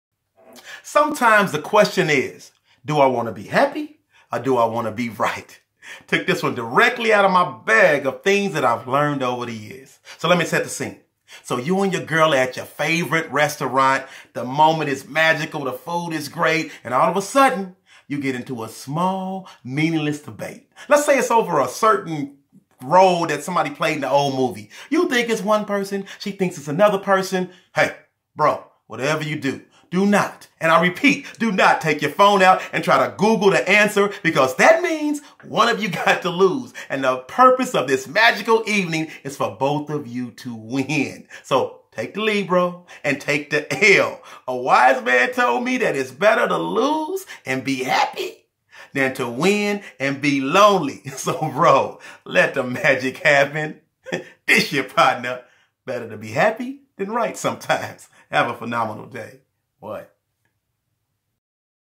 1 Minute Motivation | Happy or Right | Freddy Fri
1 Minute Motivation from motivational speaker Freddy Fri

This episode is designed to remind you that sometimes is better to Happy rather than be right!